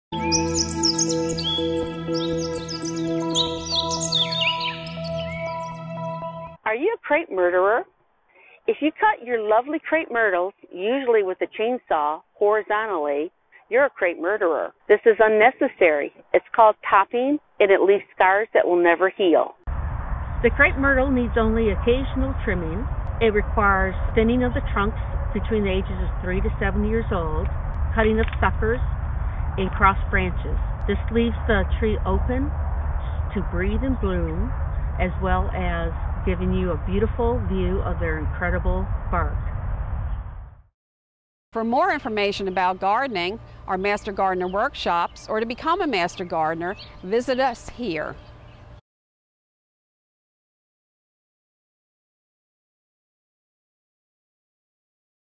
MGMinute: Don't Commit Crape Murder!
Delaware Master Gardener Beth Peterson shares some tips to properly prune this popular tree/shrub - the Crape Myrtle.